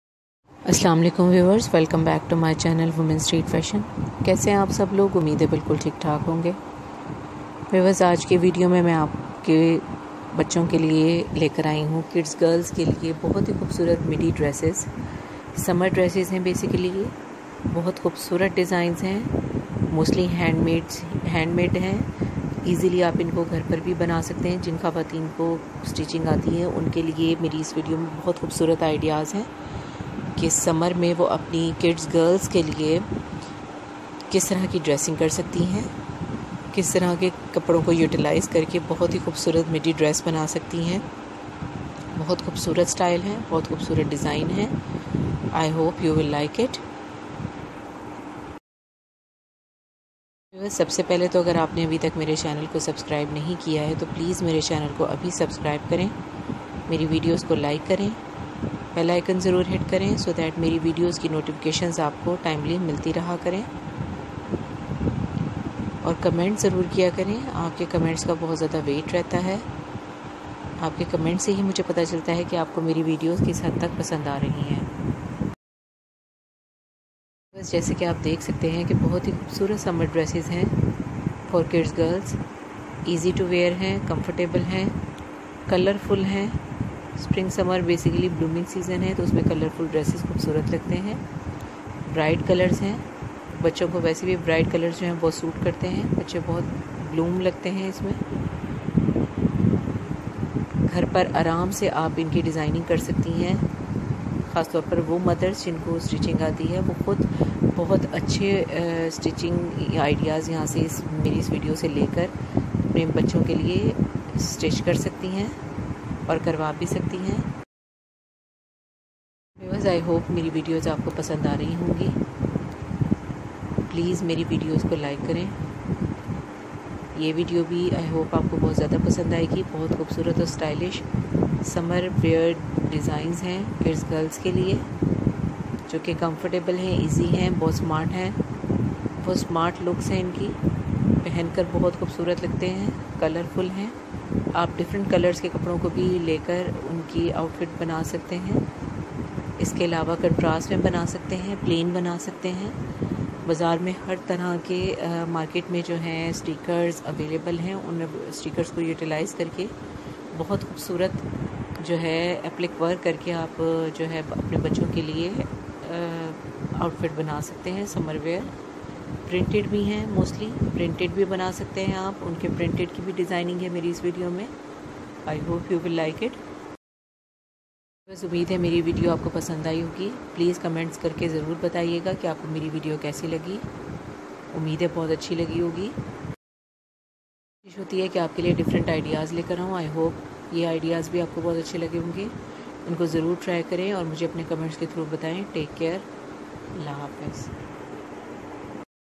Beautiful summer pinafore dress designs for kids girls/ kids girls pinafore dresses
hello viewers,  in this video I have bring for your little girls  summer drsses designs ideas.